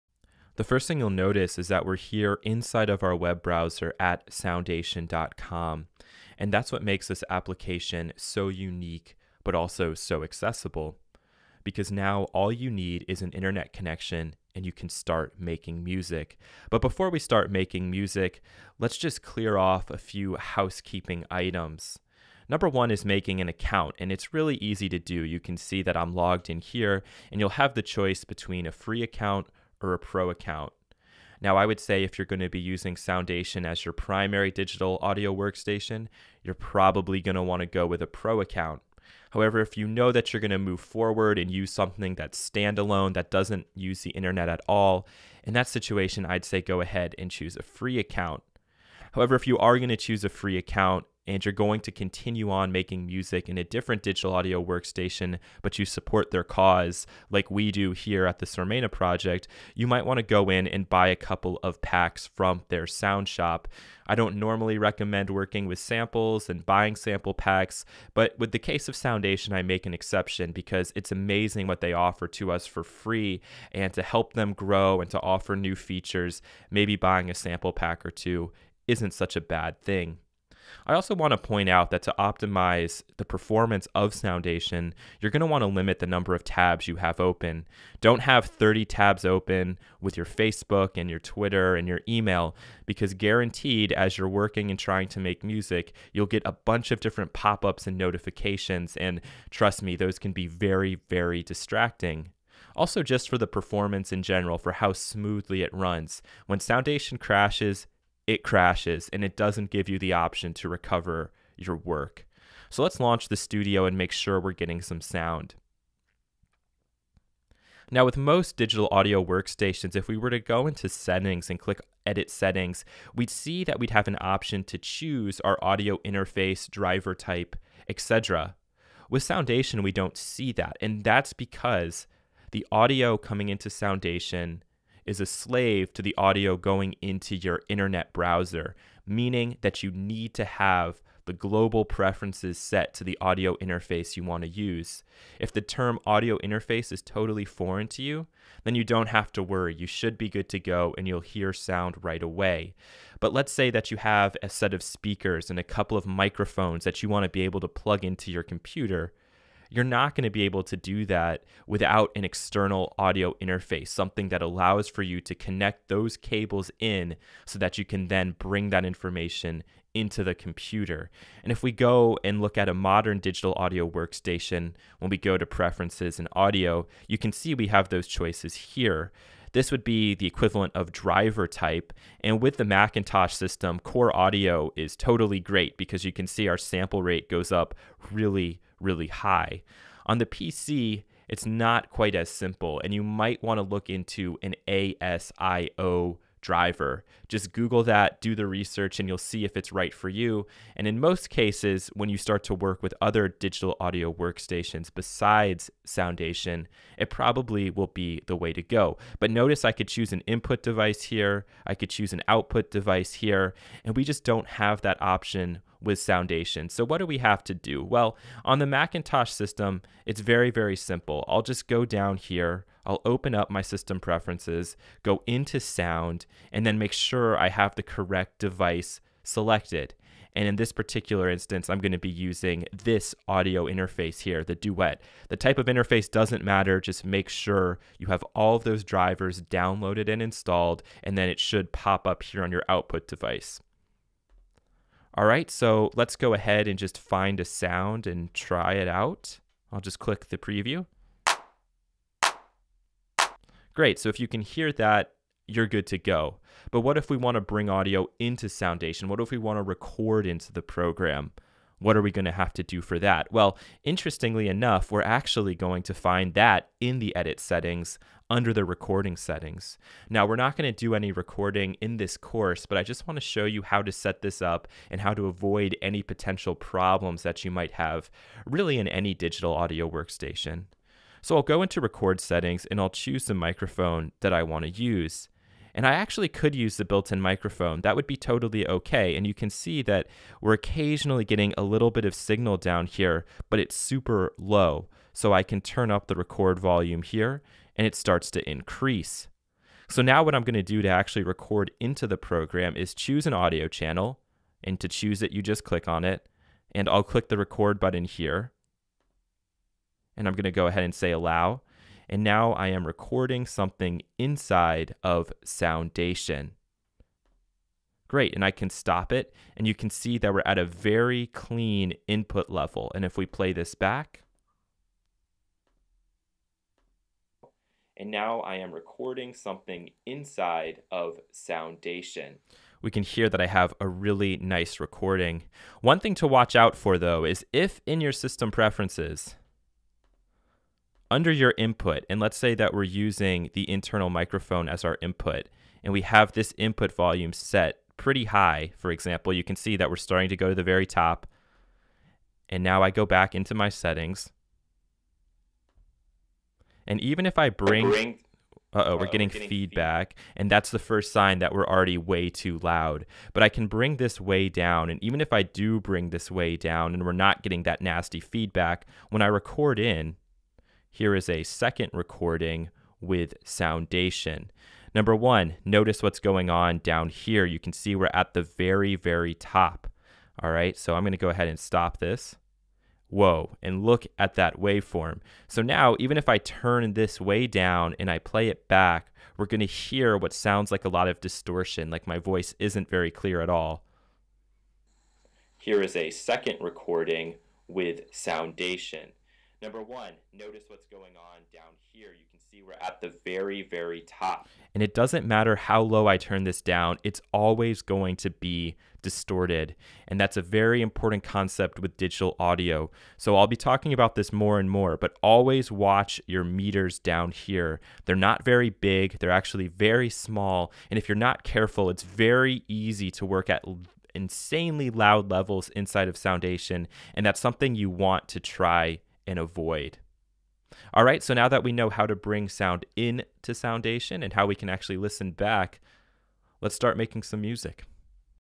Soundation - Audio In Audio Out
For higher quality (free) videos please visit Sormena.org.

There is nothing particularly exciting about scrolling through preference menus and dialogue boxes but it is an essential step if you want to actually hear what you are doing. In this video we talk about setting up an audio interface and recording into Soundation using our computers built in microphone.